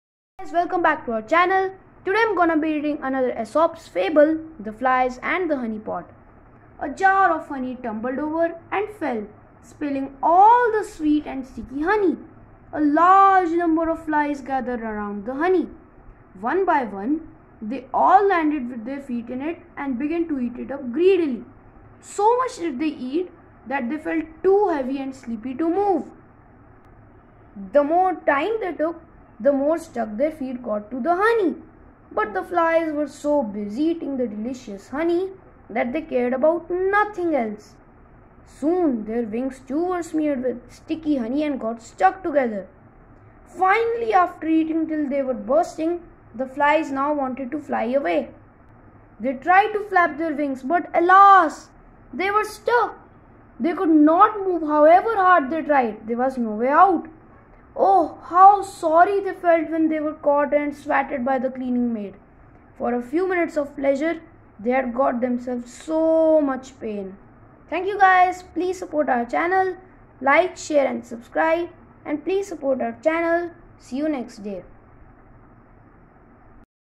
The flies and the honey pot (famous aesops fable)motivation
I have tried my best to elaborate the story. Although, it may not live up to your expectations, ee would definitely love some suggestions from you guys.